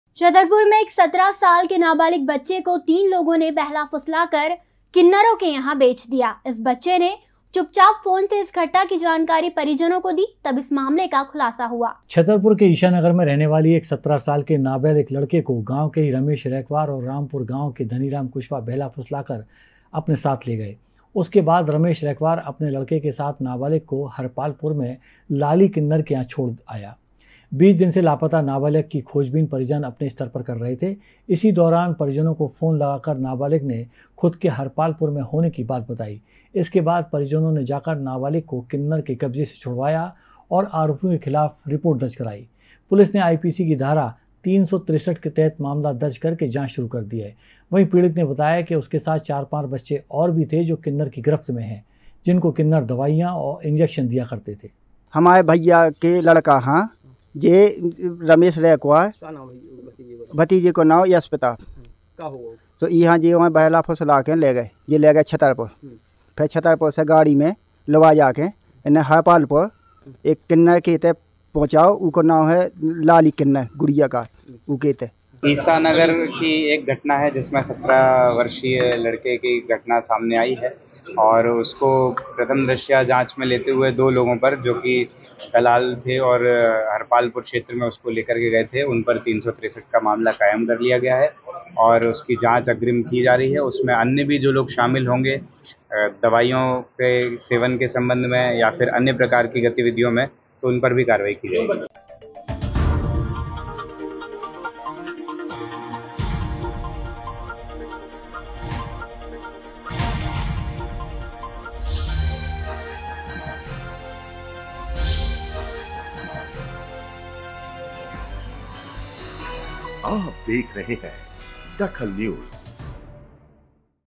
DAKHAL NEWS 17 साल के नाबालिक को किन्नरों को बेच दिया
17 साल के नाबालिक को किन्नरों को बेच दिया
नाबालिक ने फ़ोन पर दी परिजनों को जानकारी